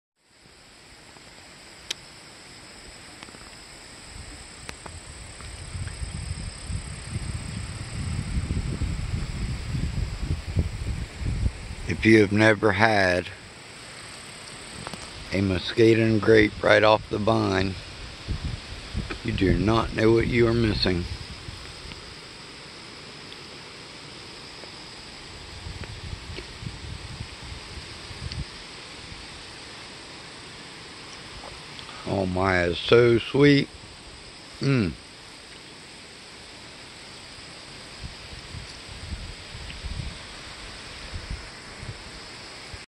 muscadine grapes on homestead
if you have never tasted a muscadine grape fresh off the vine when they are optimally ripe, you do not understand the sweetness you are missing. If you dont have these on your homestead, get them!